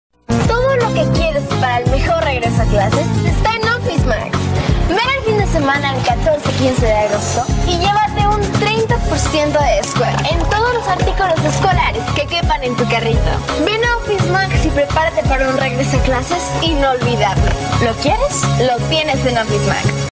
Comerciales mexicanos: Office Max 2010
Comercial del regreso a clases donde participa Danna Paola